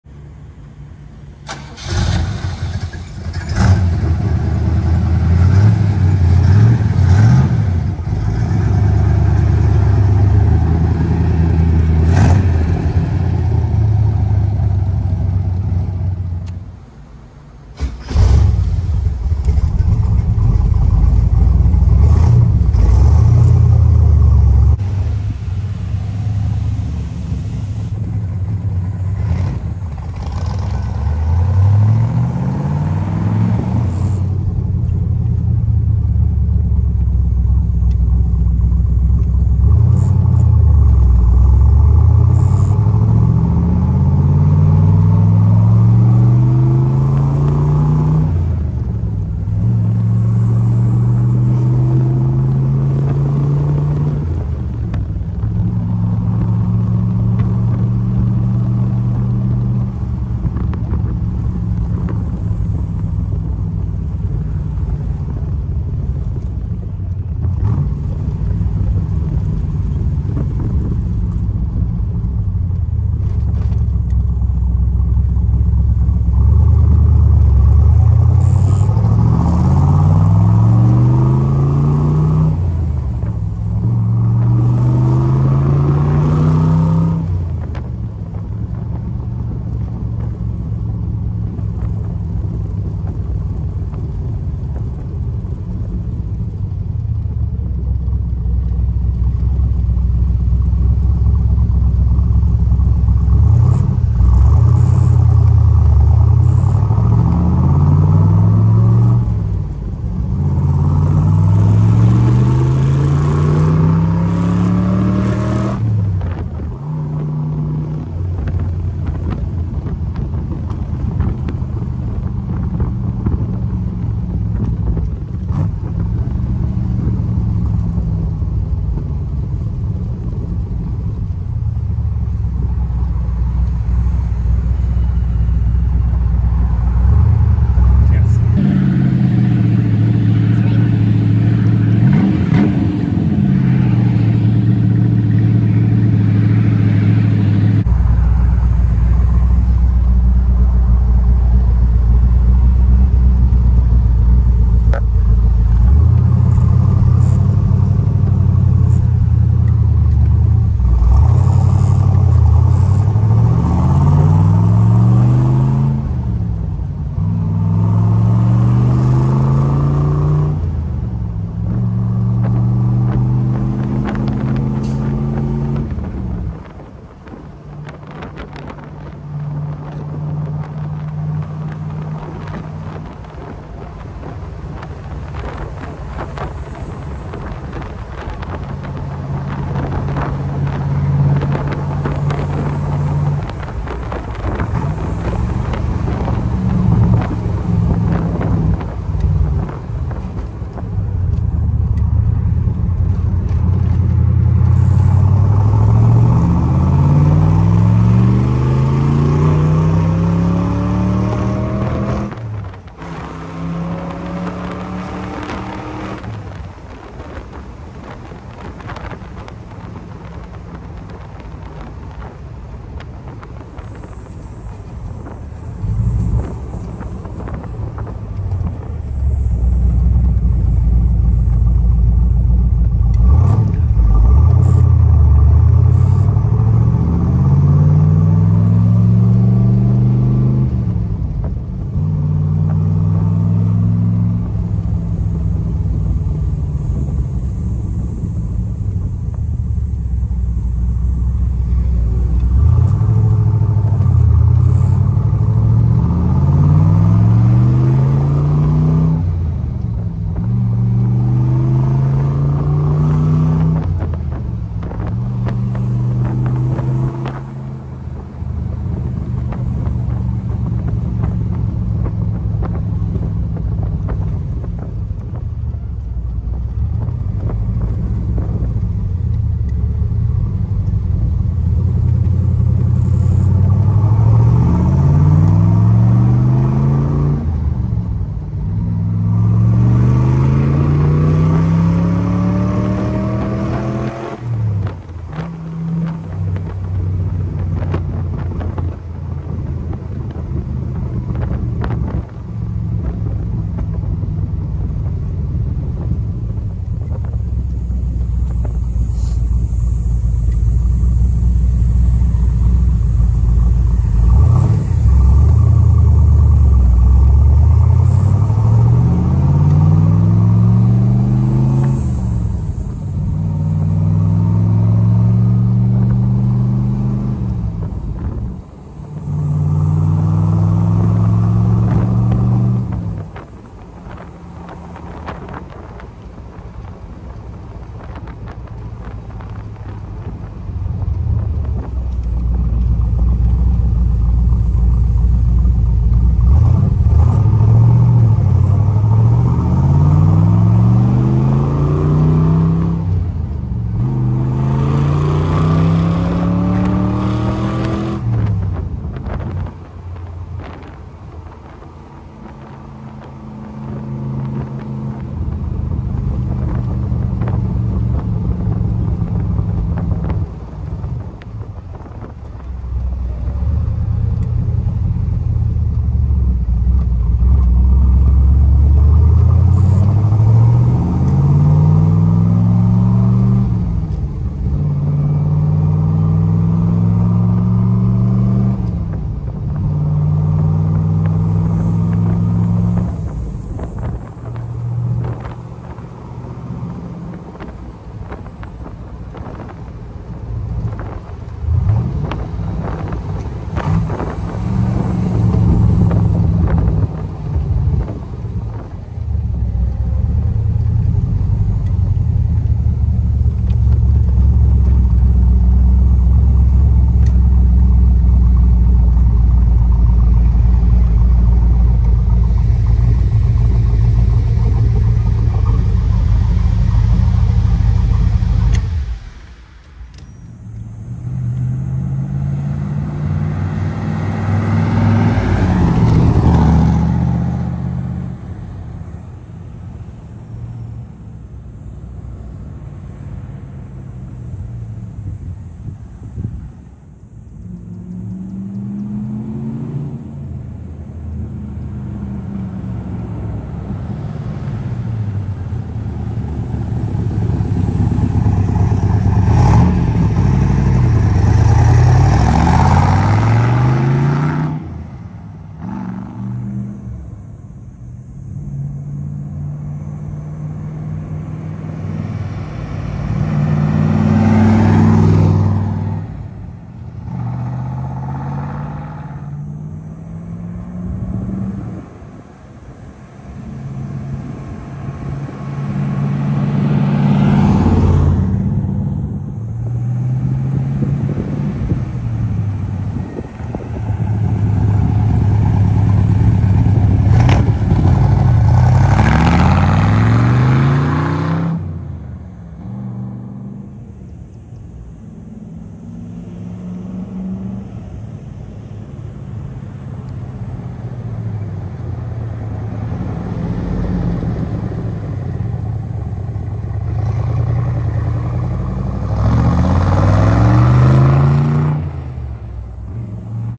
Driving Video 1965 Chevrolet Corvette Convertible L79 327ci 350hp s Match Cascio Motors
Driving video taken on 8/17/2022 of our 1965 Chevrolet Corvette Convertible powered by a numbers matching L79 327ci 350hp 4 speed. Goldwood Yellow over White interior. Selling live on Bring a Trailer auction site selling at No Reserve.

Cascio Motors
Scottsdale, AZ